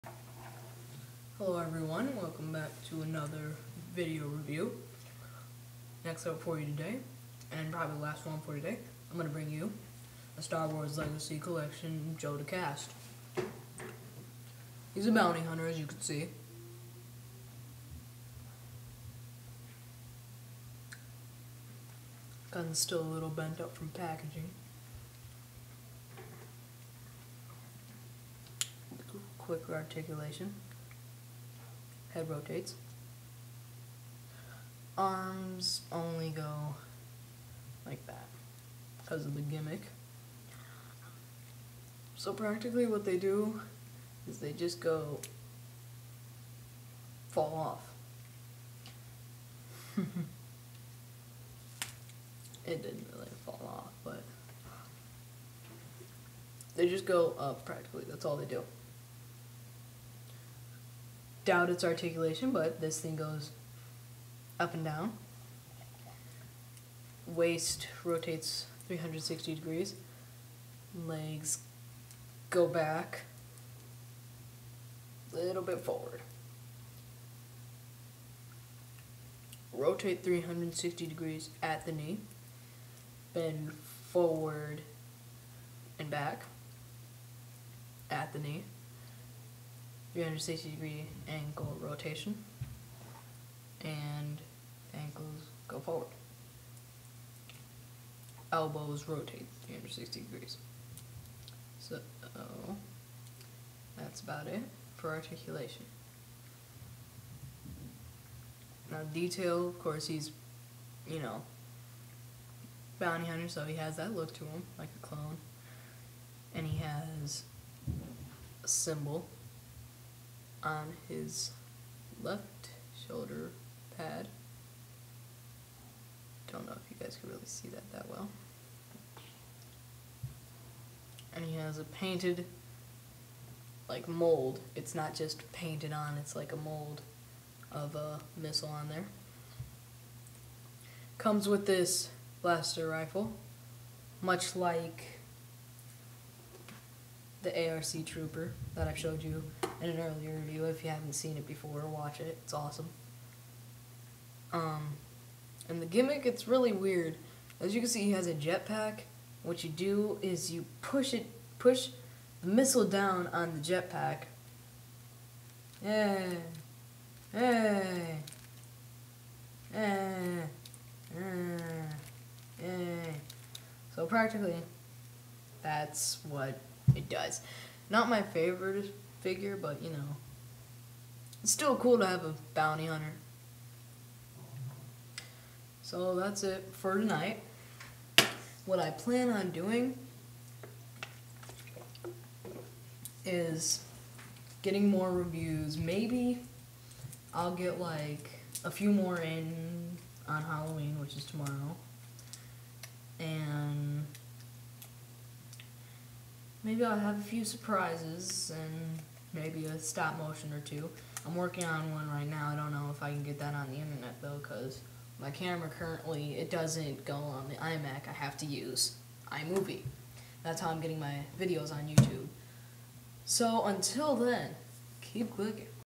Star Wars Legacy Collection Jodo Kast
My review of Jodo Kast. Love the figure. Hate the feature!